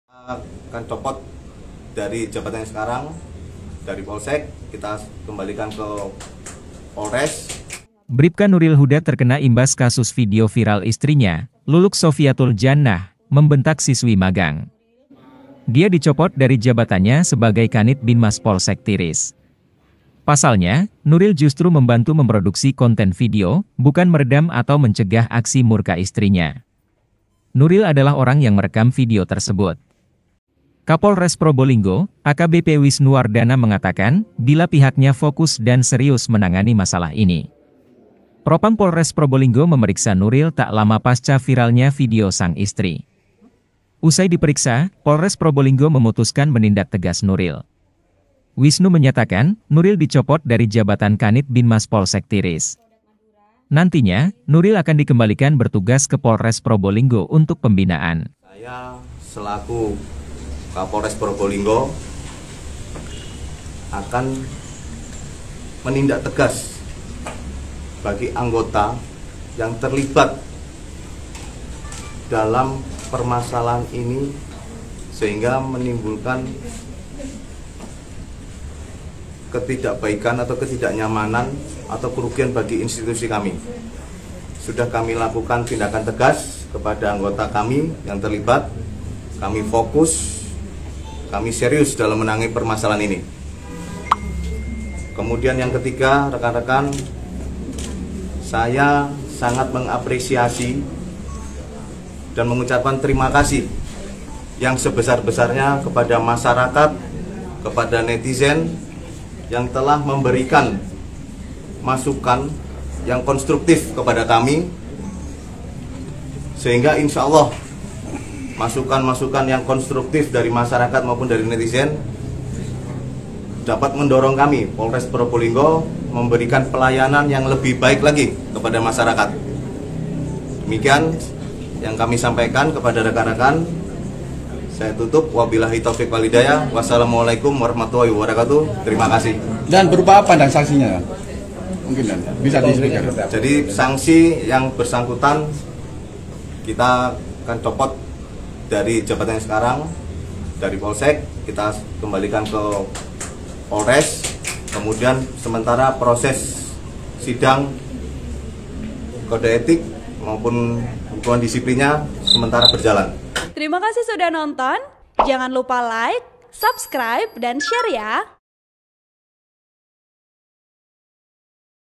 BRIPKA NURIL HUDA Kena Imbas Kasus Video Viral Sang Istri Bentak Siswi Magang, Dicopot Dari Jabatan
VP: Agus      Rep: Danendra Kusuma

TRIBUNJATIM.COM, PROBOLINGGO - Bripka Nuril Huda terkena imbas kasus video viral istrinya, Luluk Sofiatul Jannah, membentak siswi magang.

Dia dicopot dari jabatannya sebagai Kanit Binmas Polsek Tiris.

Pasalnya, Nuril justru membantu memproduksi konten video bukan meredam atau mencegah aksi murka istrinya.

Nuril adalah orang yang merekam video tersebut.

Kapolres Probolinggo, AKBP Wisnu Wardana mengatakan bila pihaknya fokus dan serius menangani masalah ini.

Propam Polres Probolinggo memeriksa Nuril tak lama pasca viralnya video sang istri.

Usai diperiksa, Polres Probolinggo memutuskan menindak tegas Nuril.